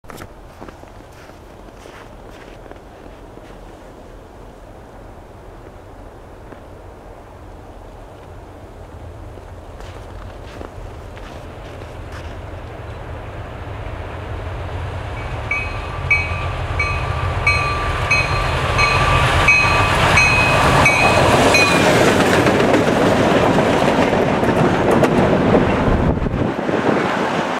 Cn 570 Cn 4726 Cn 7083 Cn 4728 at Georgetown Ontario January 14 2018
Cn 570 returning to Mac yard after running the extra train to Aldershot Mile 23.0 Halton Sub January 14 2018